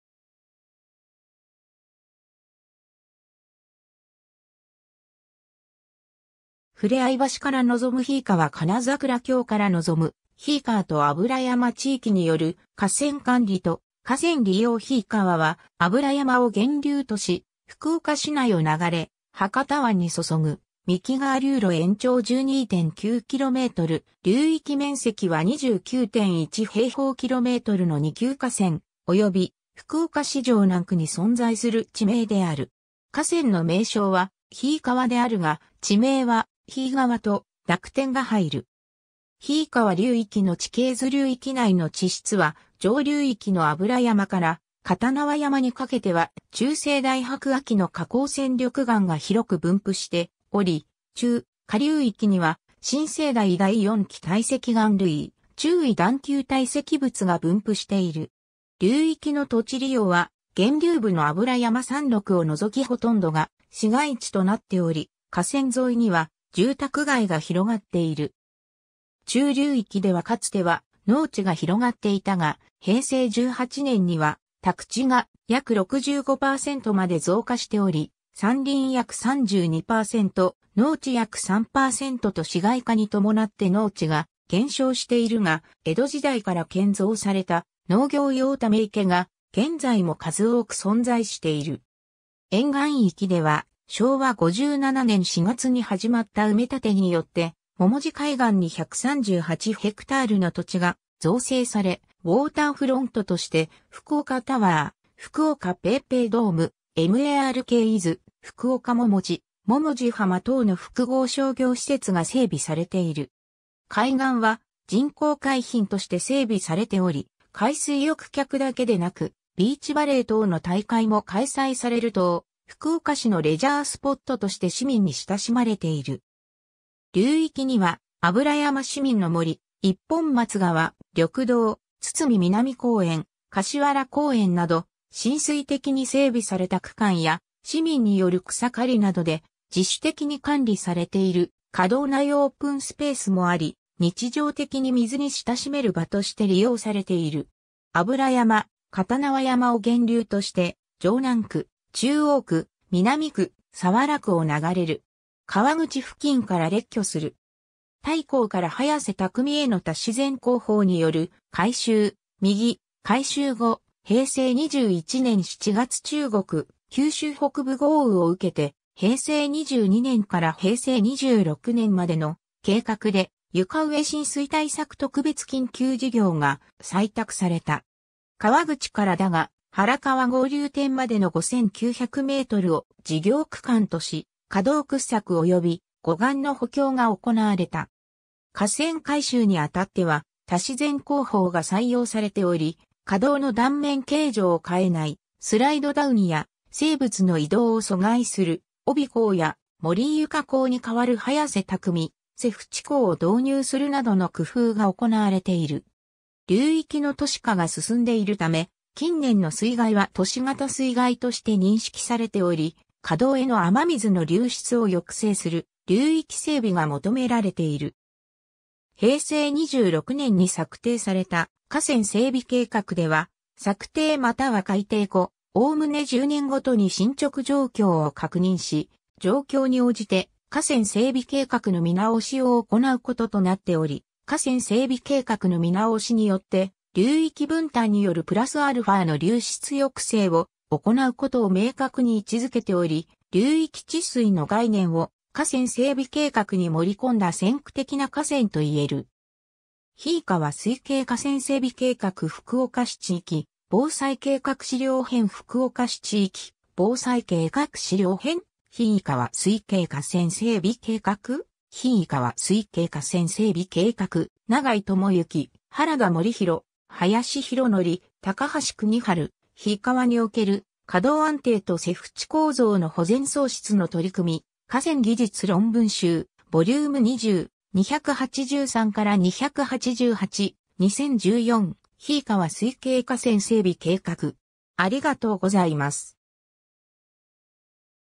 樋井川
ふれあい橋（早良区百道浜）から望む樋井川 金桜橋（城南区田島・中央区笹丘）から望む樋井川と油山 地域による河川管理と河川利用 樋井川（ひいかわ、ひいがわ）は油山を源流とし、福岡市内を流れ博多湾に注ぐ幹川流路延長12.9km、流域面積は 29.1km2の二級河川および福岡市城南区に存在する地名である。
河川の名称は「ひいかわ」であるが、地名（城南区樋井川）は「ひいがわ」と濁点が入る。
 樋井川流域の地形図 流域内の地質は、上流域の油山から片縄山にかけては、中生代白亜紀の花崗閃緑岩（古期領家花崗岩類）が広く分布しており、中・下流域には、新生代第四紀堆積岩類（海成及び非海成層）、中位段丘堆積物が分布している。
流域の土地利用は源流部の油山山麓を除きほとんどが市街地となっており、河川沿いには住宅街が広がっている。
中流域ではかつては農地が広がっていたが、平成18年(2006年)には宅地が約65％まで増加しており、山林約 32％、農地約 3％と市街化に伴って農地が減少しているが、江戸時代から建造された農業用ため池が現在も数多く存在している。
 沿岸域では昭和57年 (1982年)4月に始まった埋め立てによって百道海岸に138haの土地が造成され、ウォーターフロントとして福岡タワー、福岡ペイペイドーム、MARK IS 福岡ももち、百道浜等の複合商業施設が整備されている。
海岸は人工海浜として整備されており、海水浴客だけでなく、ビーチバレー等の大会も開催される等、福岡市のレジャースポットとして市民に親しまれている。
流域には油山市民の森（水の森）、一本松川緑道、堤南公園、柏原公園など親水的に整備された区間や、市民による草刈りなどで自主的に管理されている河道内オープンスペースもあり日常的に水に親しめる場として利用されている。
 油山、片縄山を源流として城南区、中央区、南区、早良区を流れる。
 河口付近から列挙する。
 帯工から早瀬工への多自然工法による改修（左：改修前（2010年7月1日撮影），右：改修後（2014年8月17日撮影） 平成21年7月中国・九州北部豪雨を受けて平成22年から平成26年までの計画で床上浸水対策特別緊急事業が採択された。
河口から駄ヶ原川合流点までの5,900mを事業区間とし、河道掘削および護岸の補強が行われた。
河川改修にあたっては、多自然工法が採用されており、河道の断面形状を変えないスライドダウンや生物の移動を阻害する帯工や護床工に代わる早瀬工・瀬淵工を導入するなどの工夫が行われている。
 流域の都市化が進んでいるため、近年の水害は都市型水害として認識されており、河道への雨水の流出を抑制する流域整備が求められている。
平成26年に策定された河川整備計画では「策定又は改定後、概ね10年毎に進捗状況を確認し、状況に応じて河川整備計画の見直しを行う」こととなっており、河川整備計画の見直しによって，流域分担によるプラスαの流出抑制を行うことを明確に位置づけており、流域治水の概念を河川整備計画に盛り込んだ先駆的な河川と言える。
 ^ 樋井川水系河川整備計画 ^ 福岡市地域防災計画　資料編 ^ 福岡市地域防災計画　資料編 ^ 樋井川水系河川整備計画 ^ 樋井川水系河川整備計画 ^ 永井智幸、 原田守啓、 林 博徳、 高橋邦治、 樋井川における河道安定と瀬淵構造の保全創出の取り組み、河川技術論文集、vol.20、 283-288、 2014 ^ 樋井川水系河川整備計画